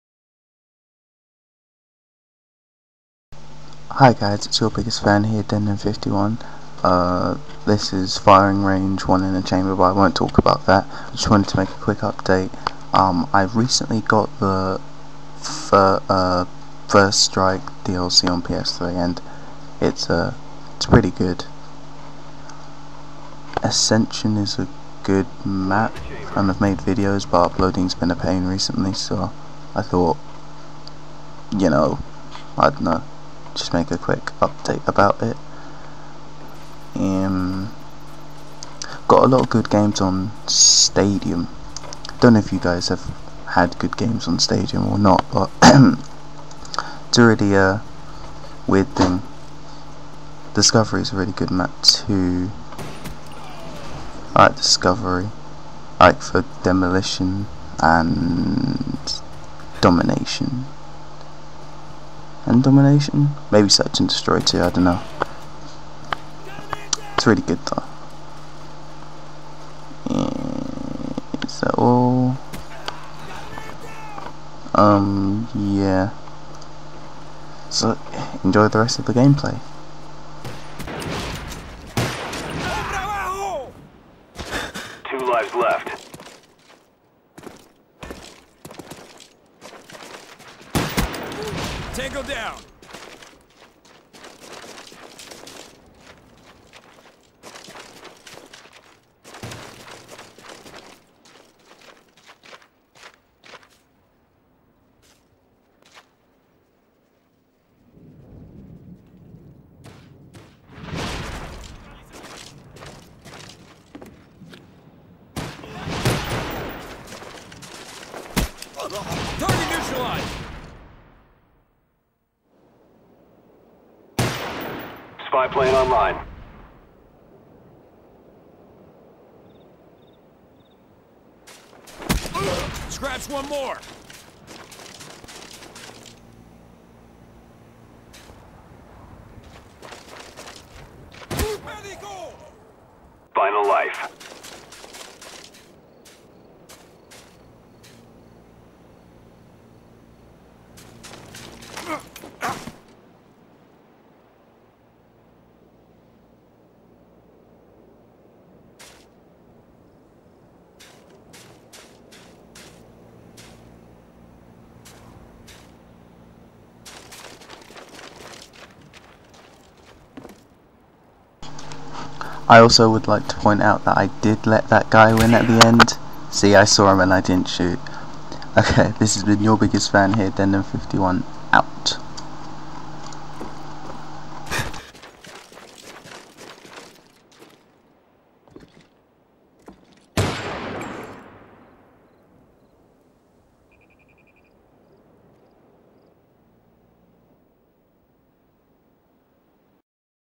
Quick update from denden51 :) (Gameplay/commentary)
Hey waddup guys, havn't uploaded a video in some time and i am failing to see what to talk about at the moment. (not really, im just acting a bit lazy)

But there is also the fact that my upload speed has recently been sucky!